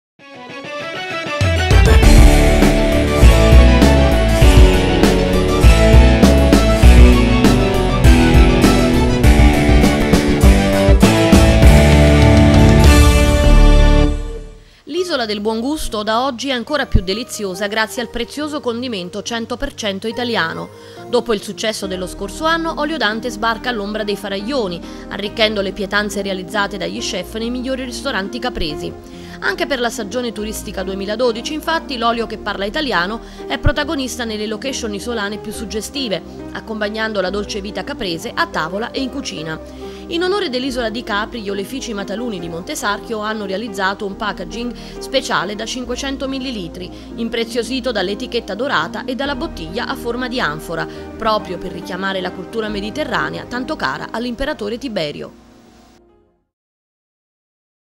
ELLE TV - Capri: Dante sbarca sull'isola del buon gusto
L'isola del buon gusto da oggi è ancora più deliziosa grazie al prezioso condimento "100% Italiano".
Dopo il successo dello scorso anno, Olio Dante sbarca all'ombra dei Faraglioni, arricchendo le pietanze realizzate dagli chef dei migliori ristoranti capresi.
Anche per la stagione turistica 2012, infatti, l'olio che parla italiano è protagonista nelle location isolane più suggestive, accompagnando la dolce vita caprese a tavola e in cucina.
In onore dell'Isola di Capri, gli Oleifici Mataluni di Montesarchio (Benevento) hanno realizzato un packaging speciale da 500ml, impreziosito dall'etichetta dorata e dalla bottiglia a forma di anfora, proprio per richiamare la cultura mediterranea tanto cara all'Imperatore Tiberio.
In particolare, tra i numerosi ristoranti che hanno scelto l'insuperabile gusto di Olio Dante in cucina, la nuova bottiglia di Olio Extravergine di Oliva Dante "100% Italiano" fa bella mostra anche sui tavoli del ristorante "Aurora" della famiglia D'Alessio -- situato in via Fuorlovado, a pochi metri dalla famosa Piazzetta -- che punta a valorizzare la semplicità ed i sapori della cultura gastronomica caprese; "Da Paolino", la caratteristica terrazza sotto i limoni di Paolino De Martino -- a Palazzo a Mare, nei pressi di Marina Grande -- dove gustare piatti di pesce fresco e tipiche ricette capresi; sui tavoli de "Il Geranio" di Lucio Gaeta -- a pochi metri dai Giardini di Augusto e dalla meravigliosa Via Krupp -- che associa il lusso contemporaneo ad una cucina mediterranea tradizionale; al "Monzù", il raffinato ristorante dell'Hotel "Punta Tragara" del Direttore Paolo Federico, che dalla suggestiva via Tragara affaccia sul panorama mozzafiato dei Faraglioni.
Ottenuto esclusivamente da olive italiane, raccolte al giusto grado di maturazione, l'Olio Extravergine di Oliva Dante "100% Italiano" è il frutto di un'attenta selezione. L'armonia del gusto e del profumo è il risultato dell'equilibrio tra le intense note di fruttato verde e il sentore piacevolmente piccante delle varietà di olivo dal profilo sensoriale più deciso, tipiche di alcune aree olivicole dell'Italia meridionale, ed i sentori più dolci e delicati di altre cultivar italiane selezionate.
Un olio dal sapore esclusivo, dunque, per un'estate caprese che si annuncia particolarmente ricca di eventi mondani da condire in compagnia di amici e gourmet.